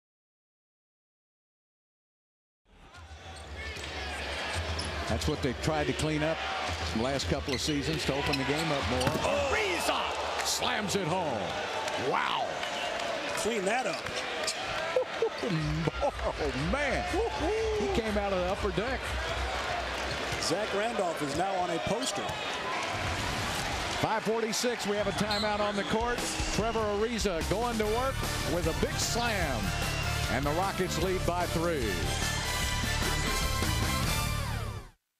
Trevor Ariza finds a seam and throws one down
Houston Rockets Trevor Ariza somehow finds a seam in the defense off of a Chuck Hayes pick and throws down this nasty slam against the Memphis Grizzlies on 2/5/2010